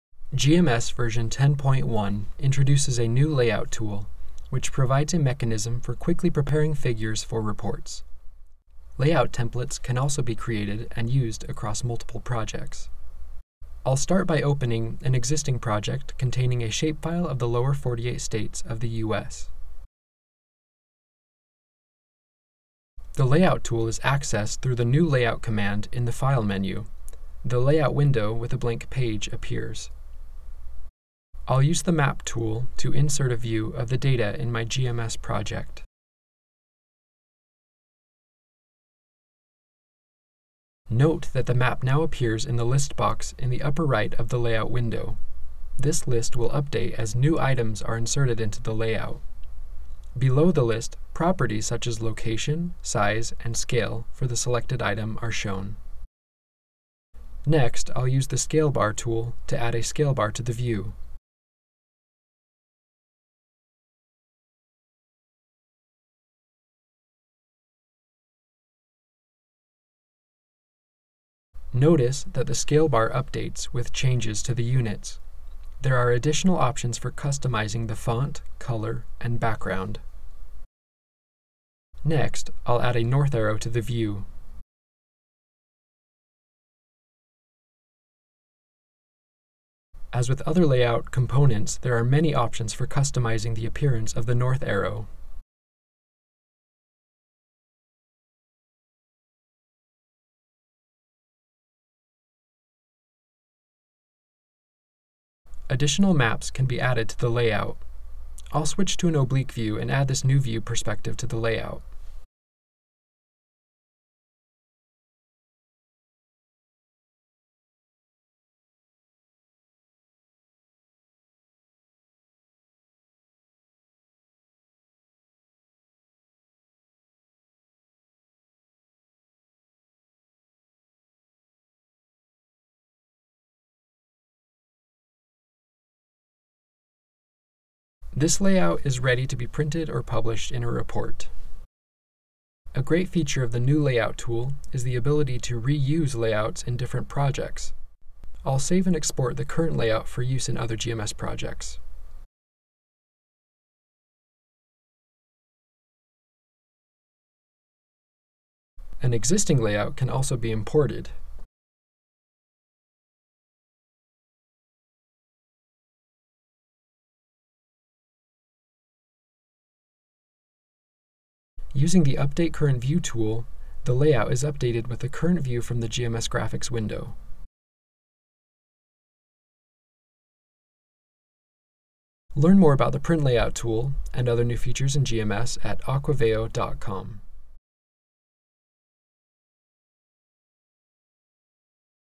GMS: Print Layout Editor
GMS version 10.1 introduces a new Layout tool, which provides a mechanism for quickly preparing figures for reports. Layout templates can be created and used across multiple projects.
(General)